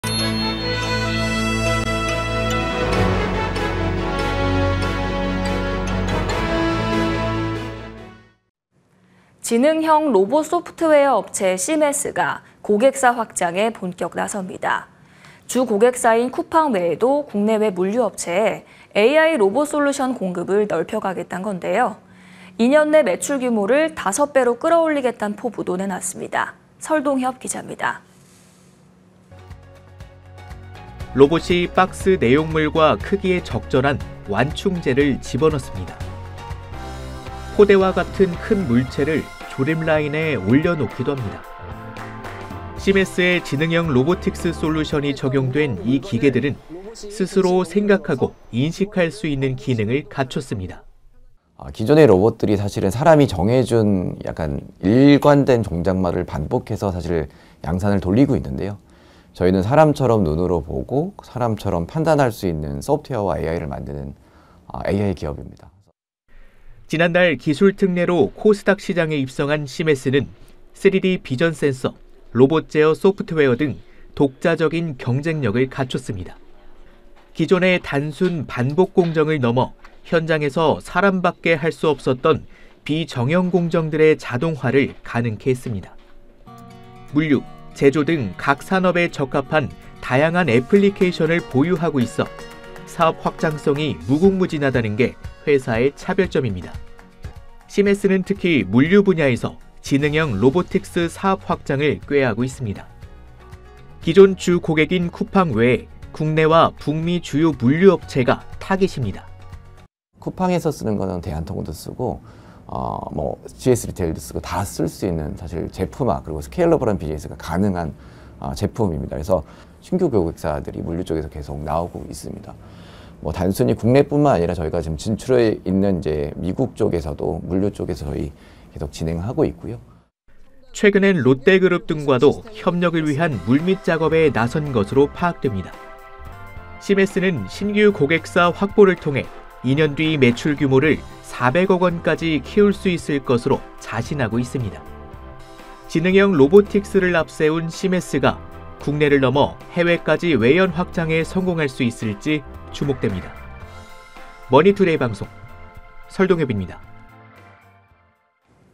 해외 확장 노리는 씨메스, 물류 AI 로봇 고객다변화 속도 [기업 INSIDE] / 머니투데이방송 (뉴스)
11/27 MTN 핫라인 5

지능형 로봇 소프트웨어 업체 씨메스(CMES)가 고객사 확장에 본격 나섭니다.

주고객사인 쿠팡 외에도 국내외 물류 업체에 AI 로봇 솔루션 공급을 넓혀가겠단 건데요.

 2년내 매출 규모를 5배로 끌어올리겠단 포부도 내놨습니다.

설동협 기자입니다.